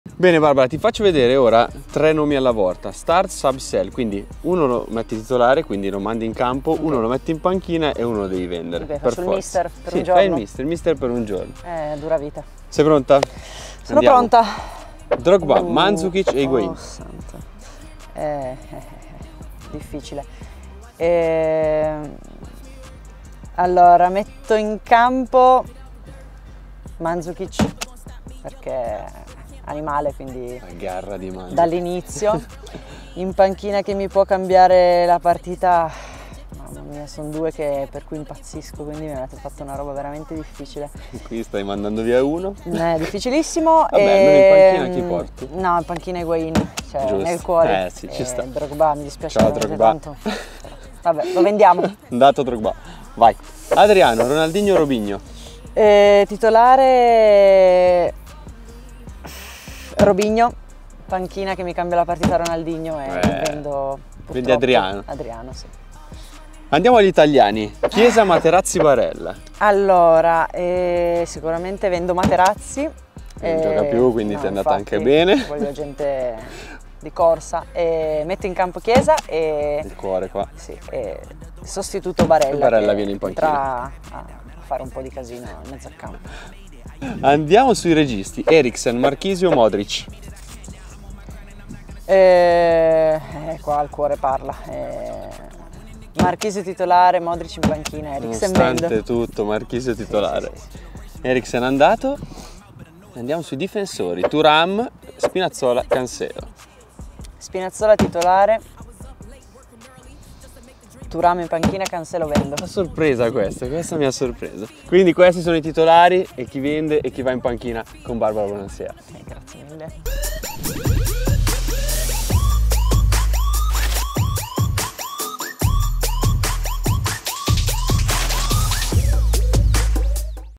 Mister per un giorno: chi schieri, chi metti in panca e chi fuori?
Questo è lo START, SUB, SELL di Barbara Bonansea
Mister for a day
This is the START, SUB, SELL of Barbara Bonansea

The Pitch Footballstore with Barbara Bonansea x @nike
Intervista registrata il 18/04/23

📍Scopri il nostro nuovo store in Corso Europa 18, Milano - M1 San Babila